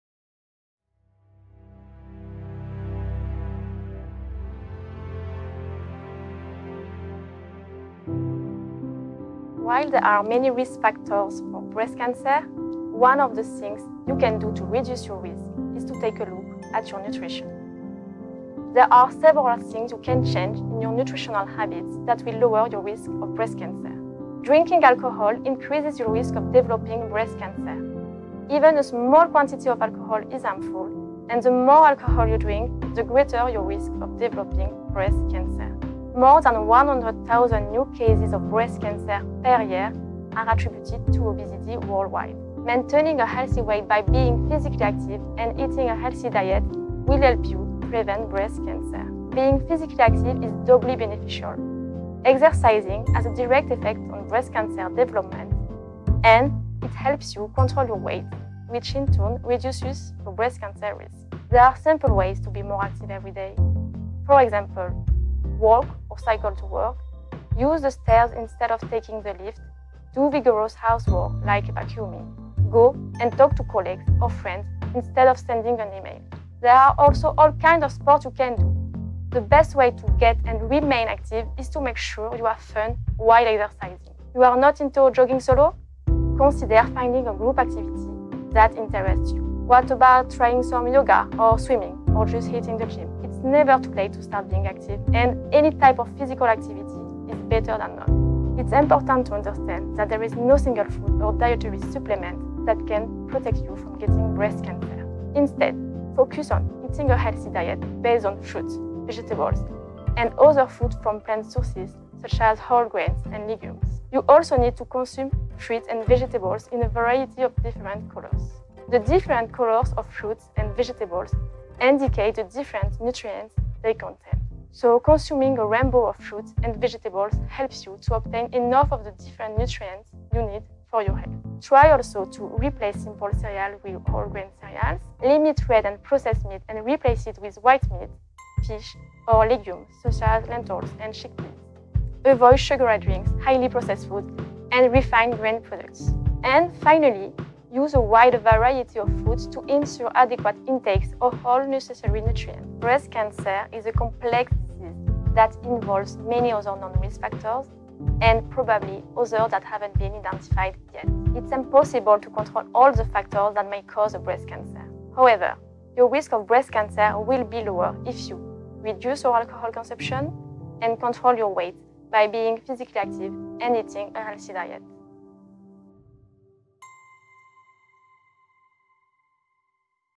Ms Manon Cairat on nutrition and the risk of breast cancer
Ms Manon Cairat, a doctoral student in the Section of Nutrition and Metabolism at the International Agency for Research on Cancer (IARC), provides dietary and lifestyle recommendations for reducing the risk of developing breast cancer, in a new video to mark Breast Cancer Awareness Month.

This video is part of a short series of interviews with IARC researchers that cover topics related to breast cancer development, risk factors, and the evidence base for public health recommendations that aim to reduce the burden of this disease.

Breast cancer is the most commonly diagnosed cancer type and the leading cause of cancer death in women globally. In 2018, there were an estimated 2.1 million new cases of breast cancer and 627 000 deaths from breast cancer worldwide.